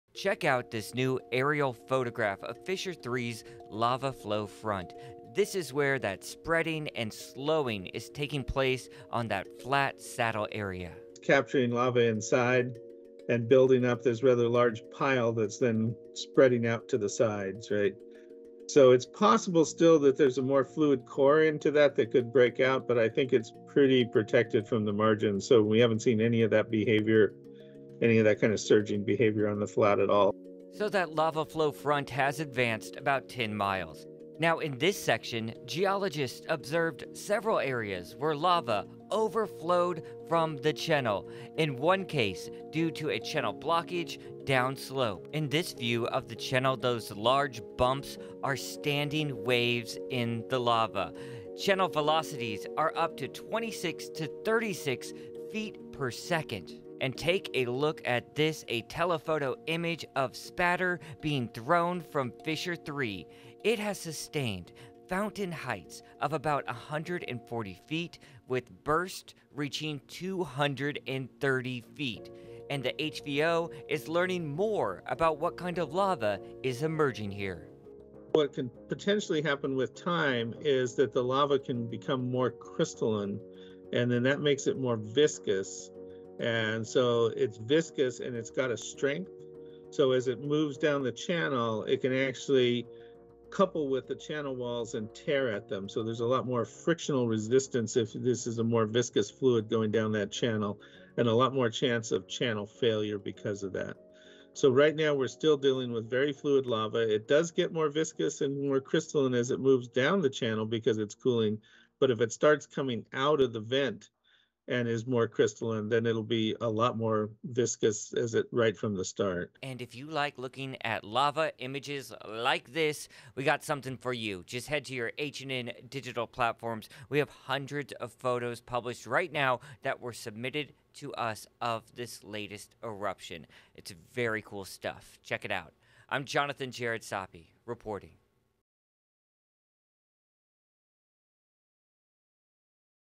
NEW PHOTOS: Here’s a look at spectacular images captured of Mauna Loa’s eruption
Hawaiian Volcano Observatory Scientist-in-Charge Ken Hon said Monday that the lava flow has “slowed down considerably” ― moving at about 20 feet per hour.